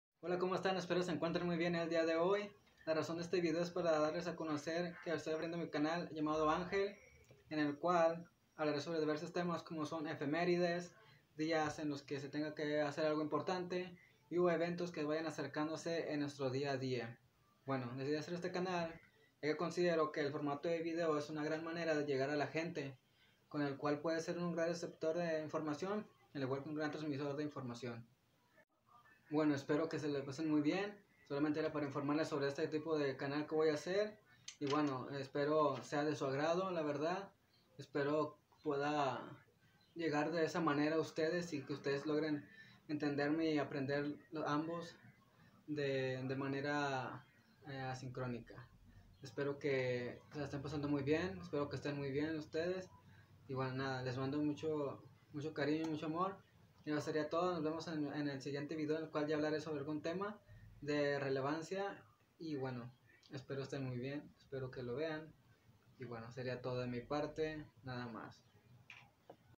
Empezando en Youtube
Decidí comenzar este canal con el fin de comunicar y dar a conocer que hay detrás de cada fecha importante o evento que se aproxime.
El siguiente video lo estaré subiendo el sábado alrededor de las 3:00 pm (horario del centro del país).
Pueden seguirme en Instagram: @angelgaytan_